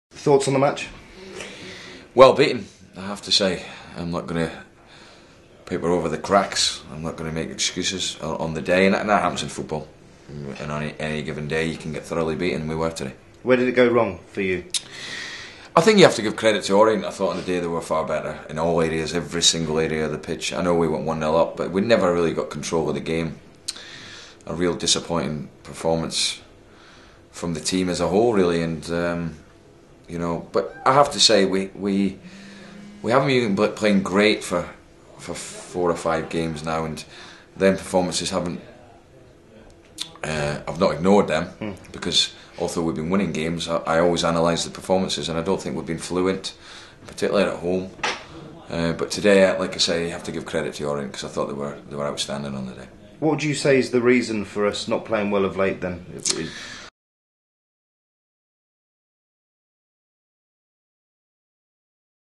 Ferguson spoke to PUTV after the 3-1 defeat to Leyton Orient at London Road.
Ferguson spoke to PUTV after the 3-1 defeat to Leyton Orient at London Road. To see more videos from Peterborough United,